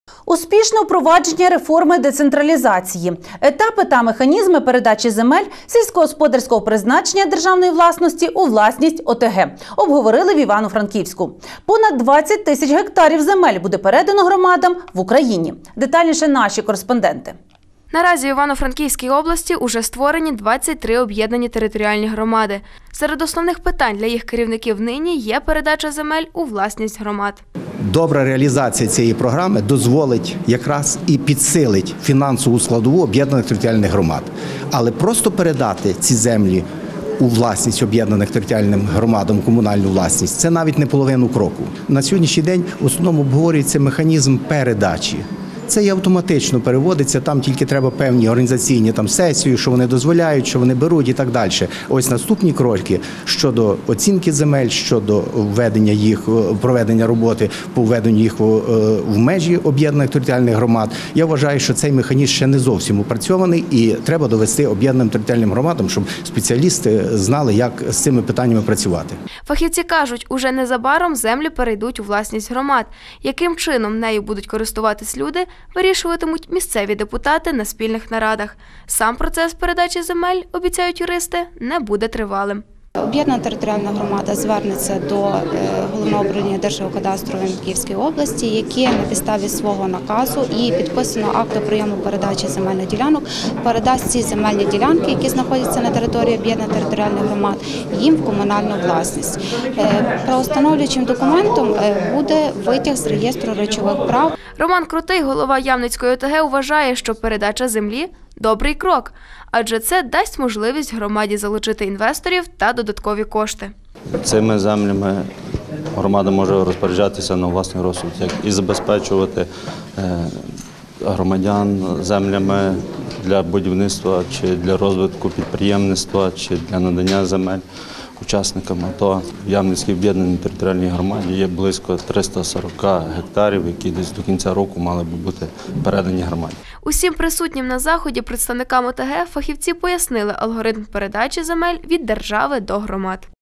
Державні землі передадуть у власність ОТГ
Успішне впровадження реформи децентралізації. Етапи та механізми передачі земель сільськогосподарського призначення державної власності за межами населених пунктів у власність ОТГ обговорили в Івано-Франківську. Понад 20 тисяч гектарів угідь буде передано громадам в Україні. Детальніше- наші кореспонденти.